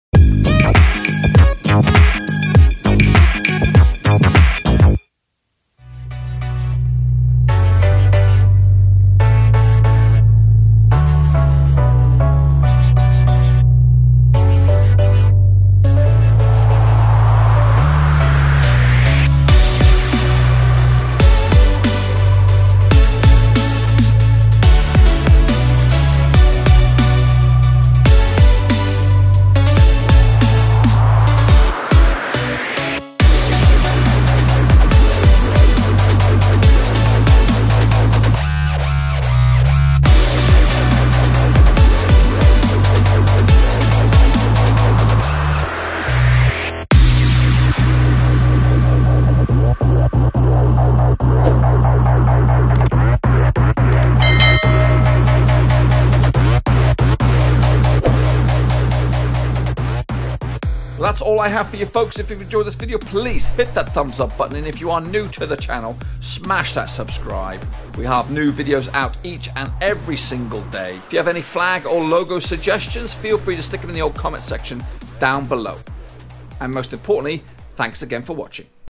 Flag of Belgium - TRANSLATE
In this video I rendered the Belgium flag on 60 second loop.

The flag is waving in the wind on a nice breezy day.

Any future suggestions for flag or logo videos are graciously accepted.

The plan for the next few months is to release one flag video per day.